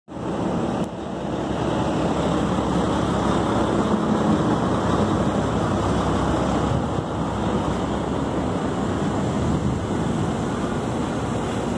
Condenado por tráfico é preso durante fiscalização da PRF na BR-226 no Tocantins
O homem que não teve o nome divulgado seguia viagem em um ônibus do transporte de passageiros. Os PRFs abordaram o veículo e ao verificar a situação dos passageiros, a equipe policial constatou que o passageiro possuía um mandado de prisão expedido pela Comarca de Morrinhos (GO), pelo crime de tráfico de drogas.